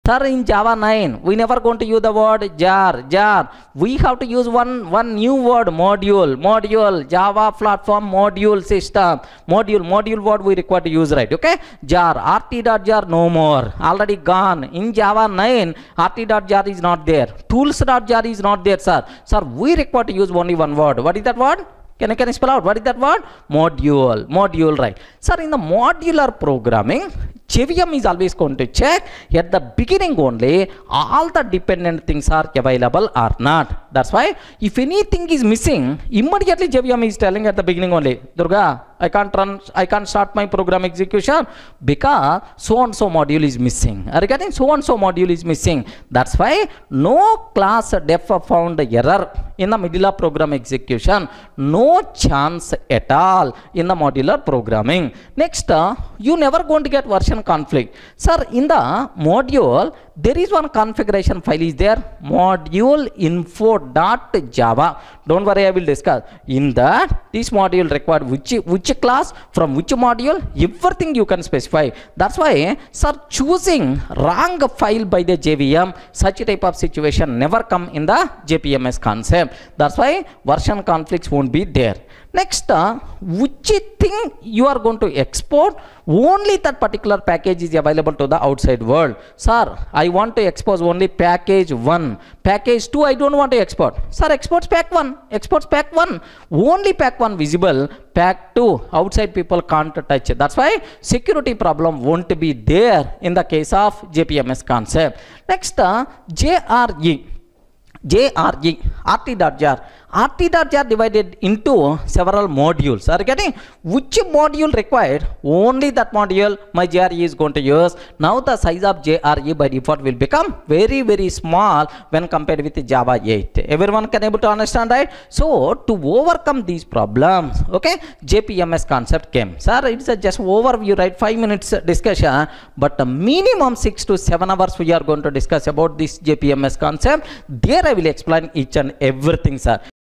Java 9 Module: A Game Changer! No More Jar/Conflicts?
Java's modular revolution arrives! Discover how the JPMS concept eliminates version conflicts and enhances security. See how smaller JREs and module-specific exports change everything. Is this the end of rt.jar?